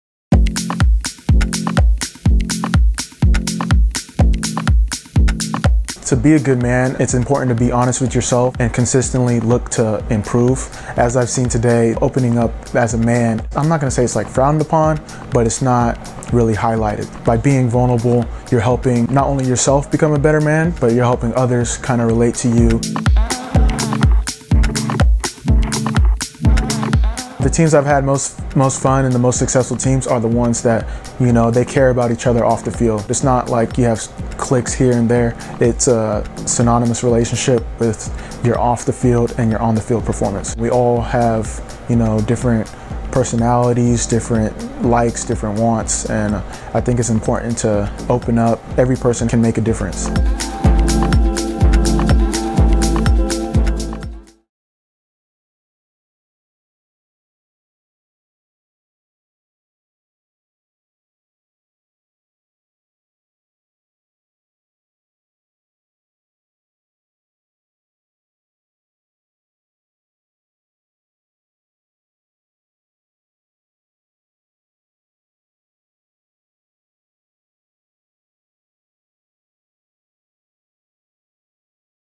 Good Men in Sport Champion: Amobi Okugo
The professional soccer player shares his thoughts on how teammates who open up and care about with each other as people also helps make better teams